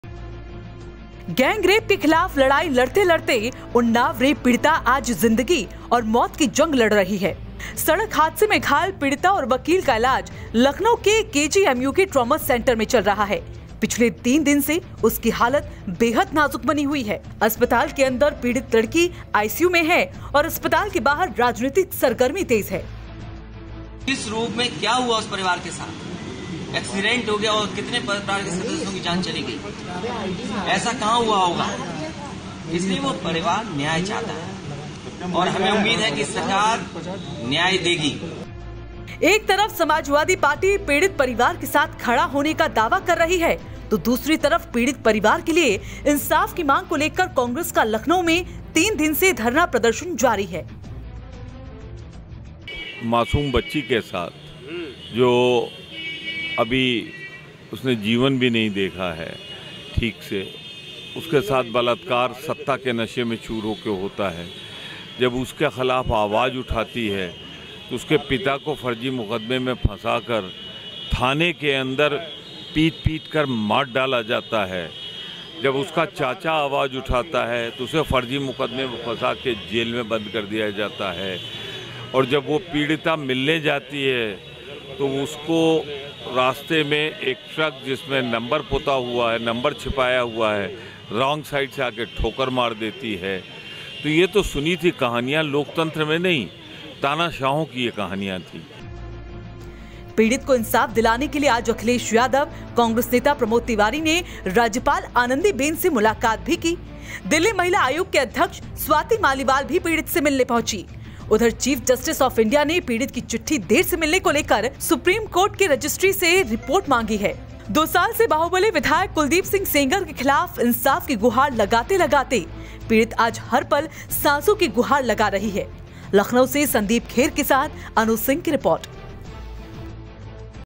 उन्नाव रेप पीड़ित की हालत नाजुक
सड़क हादसे में घायल हुई उन्नाव रेप पीड़ित अभी भी वेंटिलेटर  पर है.. अखिलेश यादव, स्वाति मालिवाल, डिप्टी सीएम केशव प्रसाद मौर्य ने की पीड़ित से मुलाकात